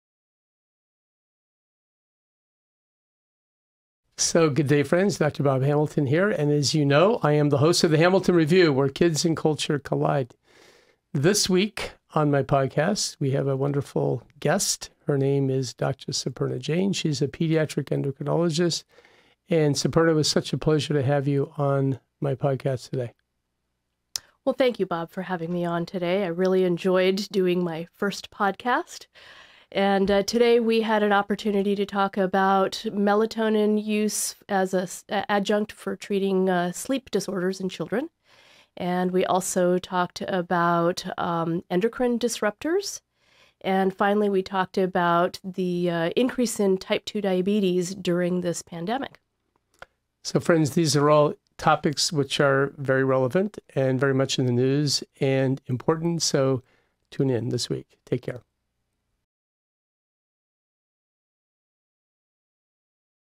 HR Ep. 169: Dr. Suparna Jain on Melatonin Use in Children, & the Increase in Type 2 Diabetes
Dr. Suparna Jain, a pediatric endocrinologist, sits down with Dr. Bob for an in depth conversation about melatonin use as an adjunct for treating sleep disorders in children, endocrine disrupters and the increase in Type 2 diabetes during the pandemic.

Don't miss this informative episode!

Listen HERE!